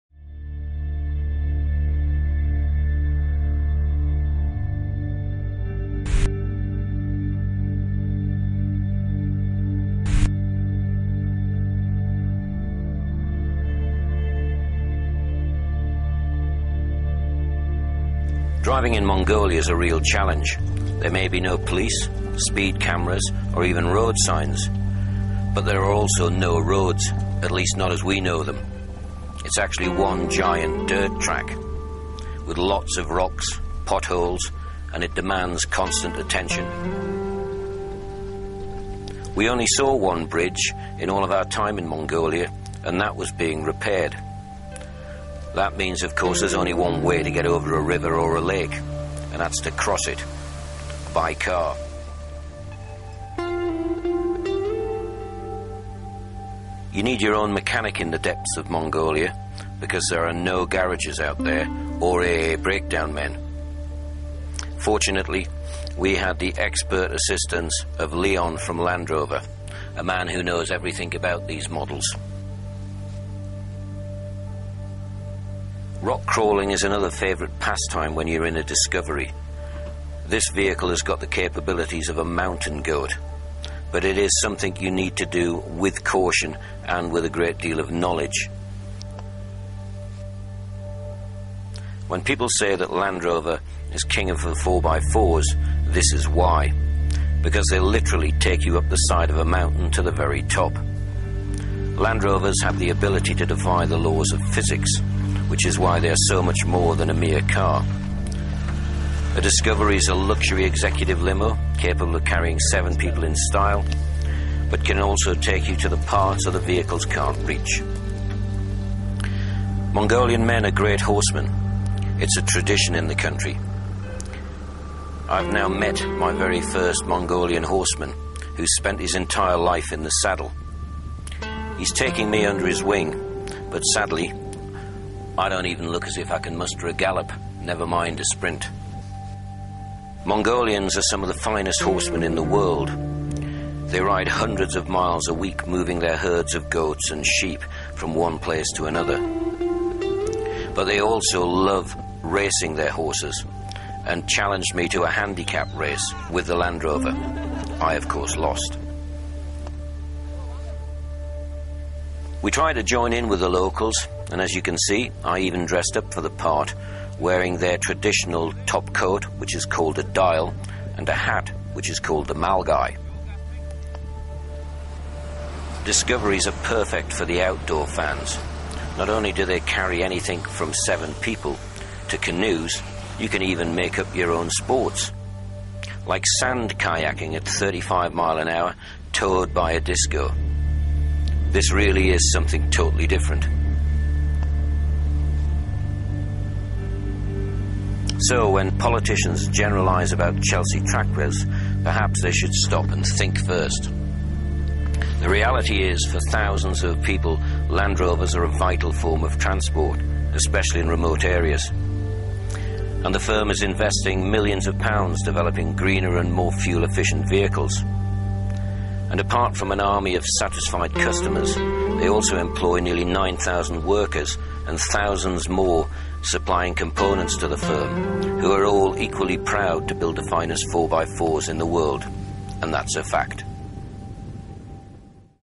Land Rover Challenge in Mongolia
Ken Gibson puts a Discovery through its paces